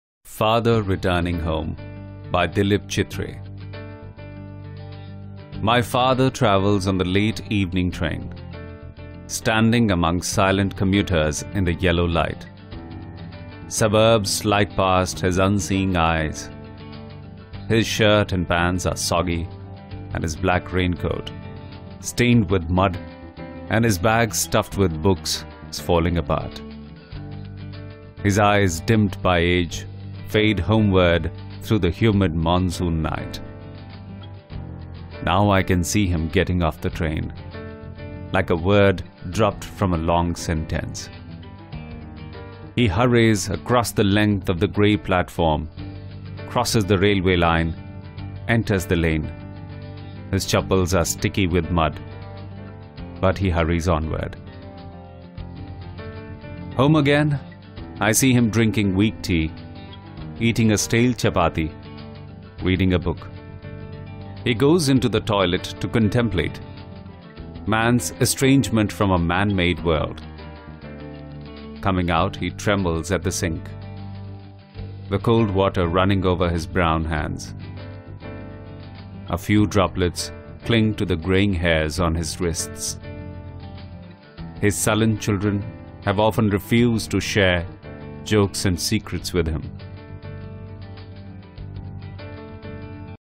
Father Returning Home by Dilip Chitre | English Literature | Class 12
Animated rendition of the popular English Literature Poetry - 'Father Returning Home' by Dilip Chitre.